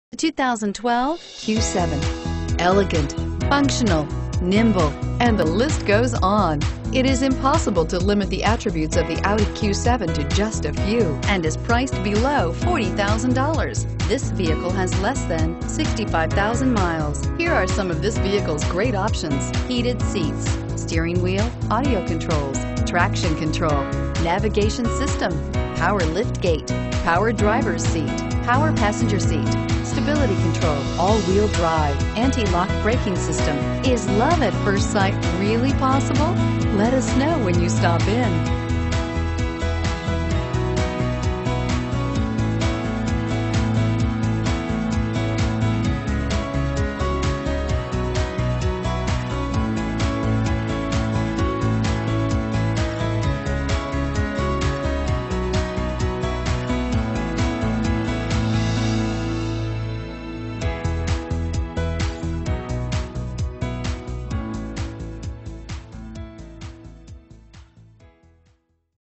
2012 Audi Q7 Puyallup WA P2361
2012 Audi Q7 3.0T Premium Plus Quattro

2001 N. Meridian

Q7 Sport Utility, 3.0L TFSI V6 DOHC, quattro...Premium Plus Package...Audi Navigation plus.